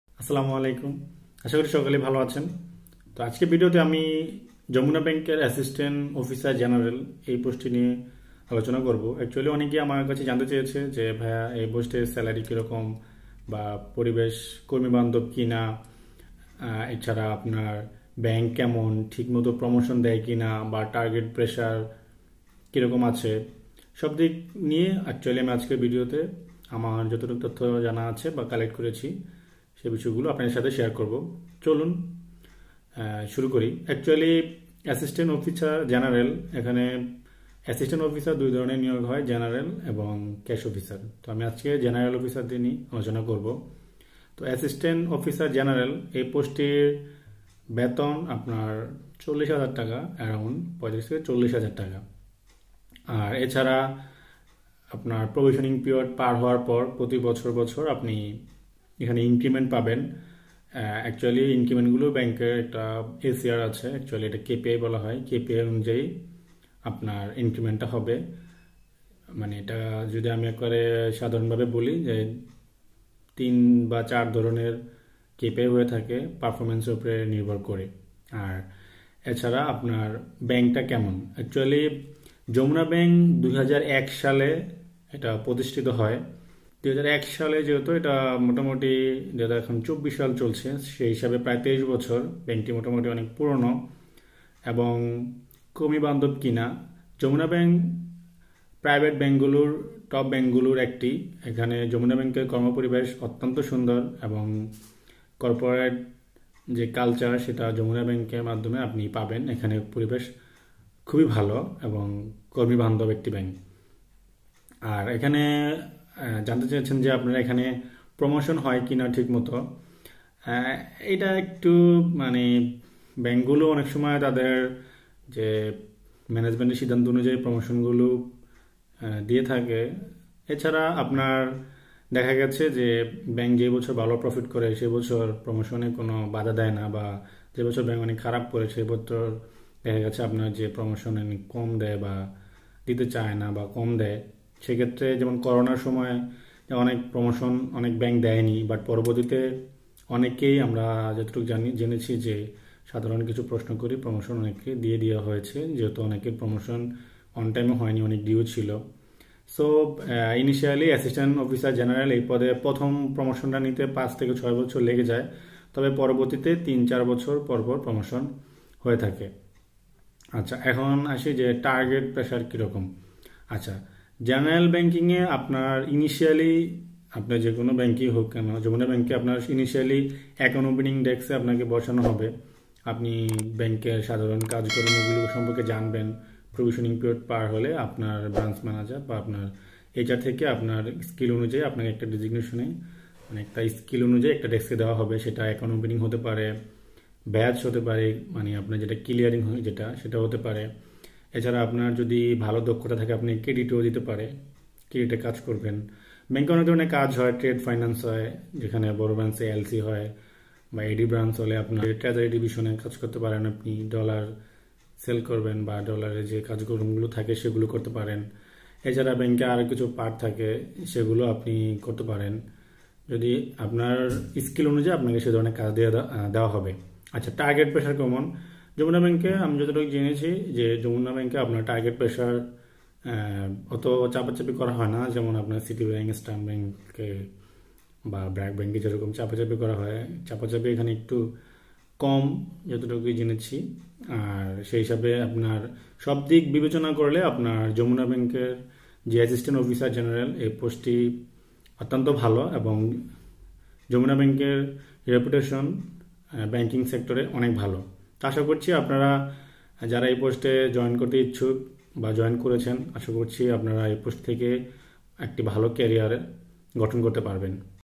Assistant Officer General Jamuna Bank
In this video I"ll share assistant officer general jamuna bank job details. Jamuna bank assistant cash Officer. Jamuna bank assistant officer.